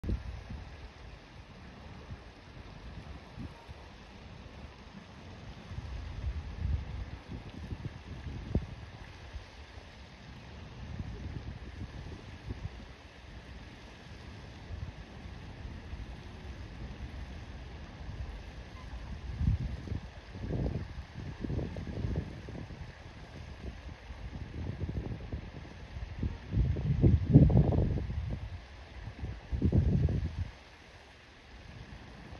滋賀っ子たちのうみのこ号と琵琶湖花噴水の虹✨🌈
滋賀県大津市浜大津港付近にて（2024年1月5日撮影）
お仕事帰りの浜大津港に滋賀の子どもたちが小学生時代に琵琶湖を旅する『うみのこ』号が停まっていました。波に揺られながら琵琶湖の上で宿泊体験を学び近江牛のカレーなどもいただいてとても素敵な船旅です。ちょうど正午の花噴水は太陽を浴びて虹がかかっていました✨